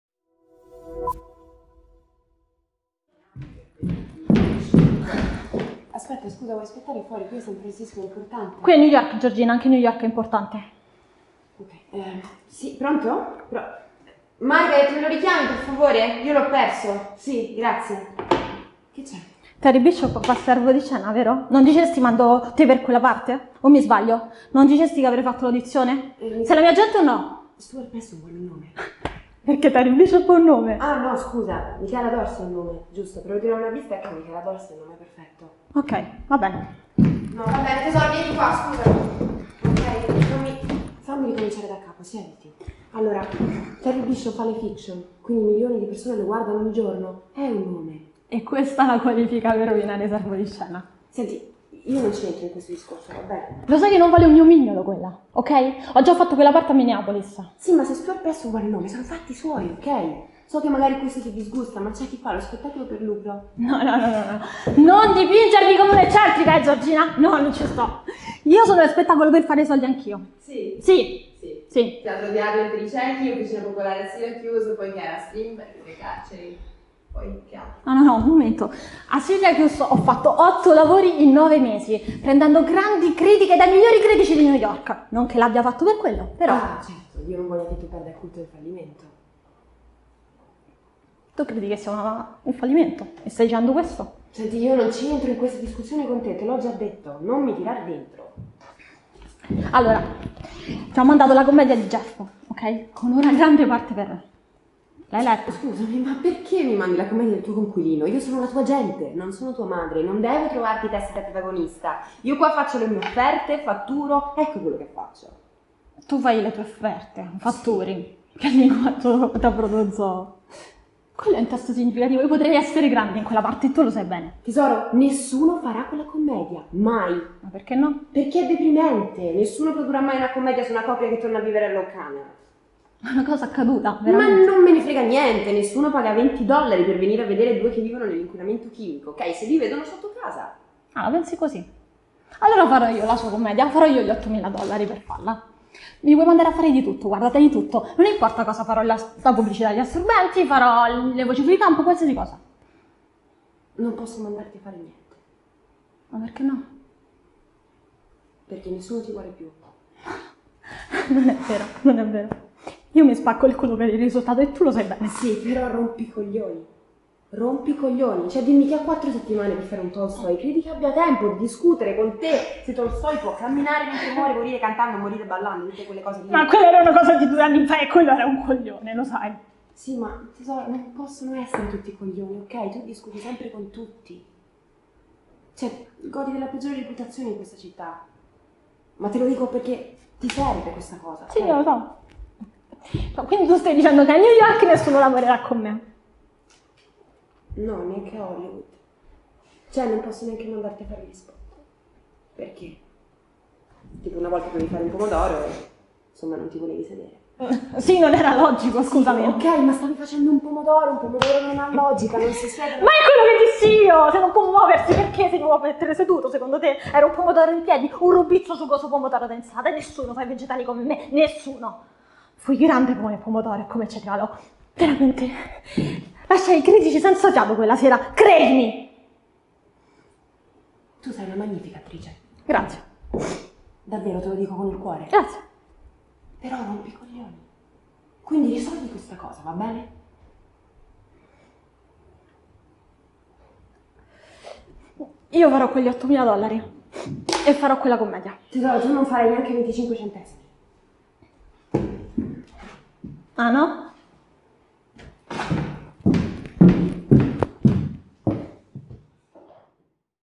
Maria Laura Moraci. Scena intera. Stage presso Professione Artista.
Maria Laura Moraci. Scena intera. Stage presso Professione Artista con Francesco Amato e Stefania Valestro.